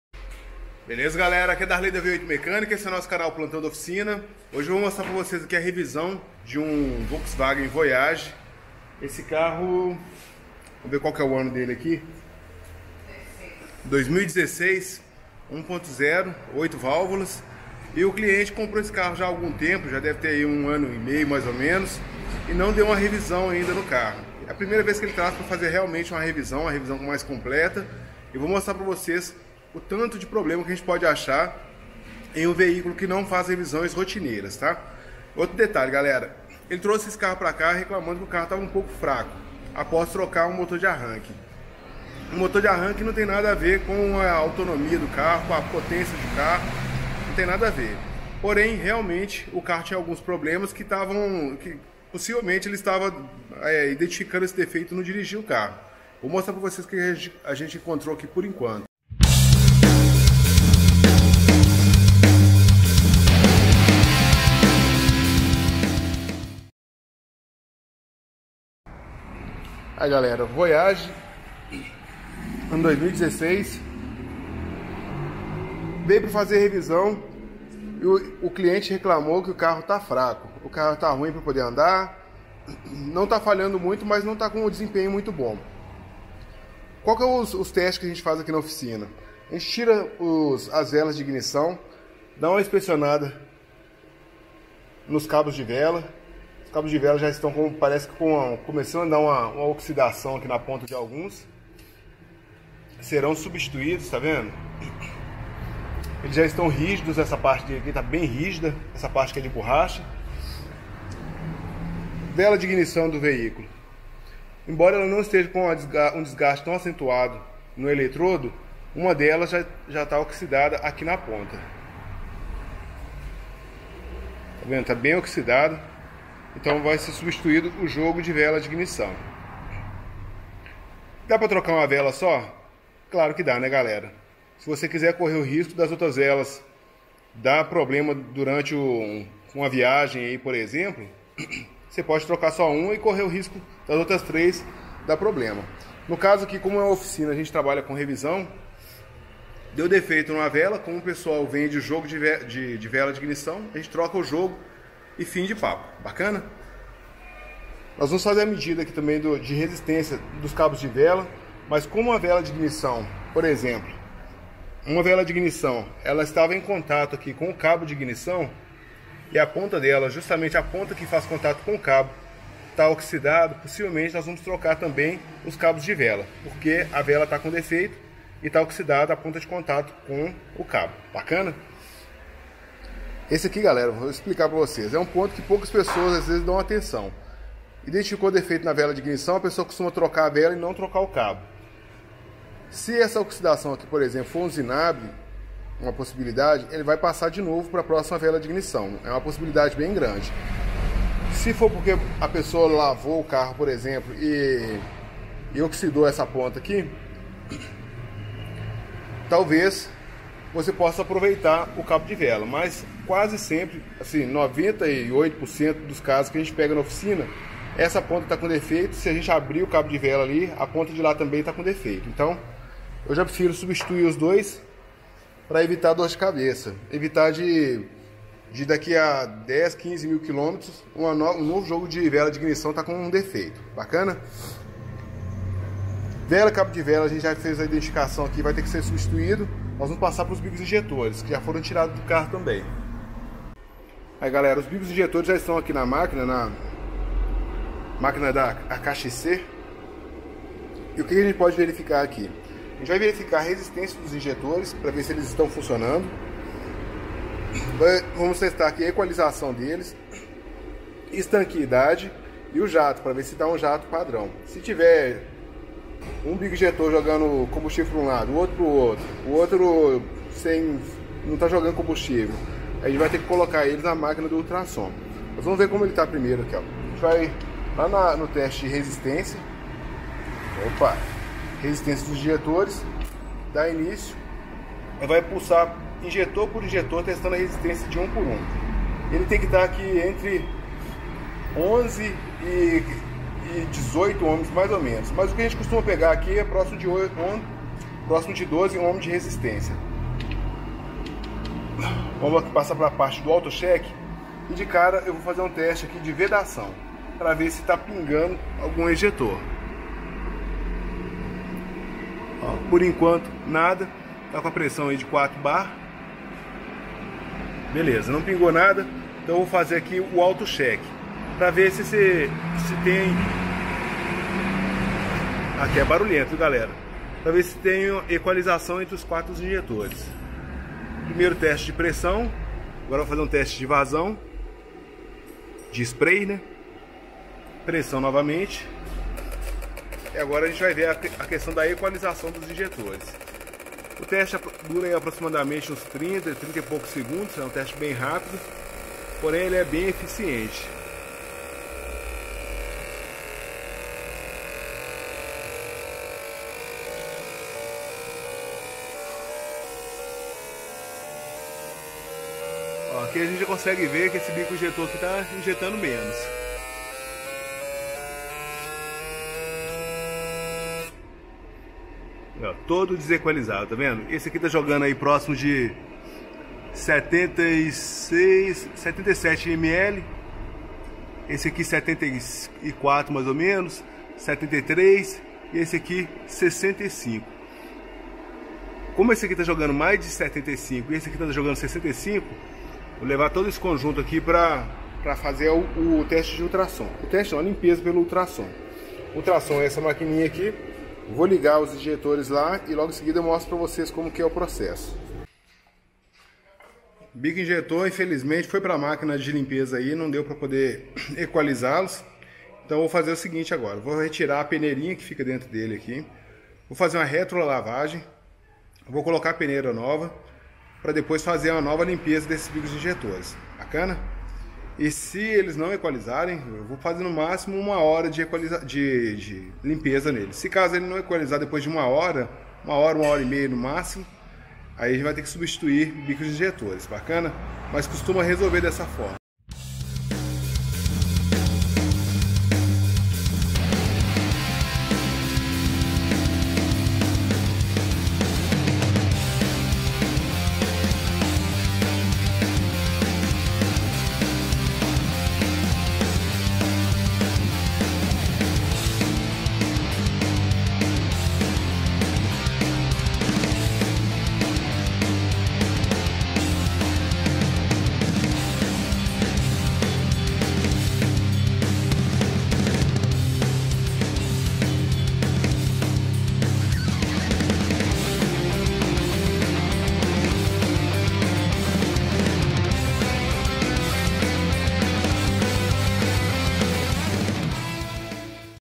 VW Voyage com defeito de injeção e ignição. ((video incompleto infelizmente))
VW Voyage com vários defeitos de injeção eletrônica e ignição. infelizmente o vídeo ficou um incompleto, pois, o cliente não quis prosseguir com os testes.